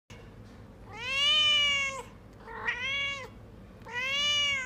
Cat sound to attract cats. realistic multiple meows/kitten - funny cats
All our cats come to this sound! Please leave comment how his loudly meows work with your cats!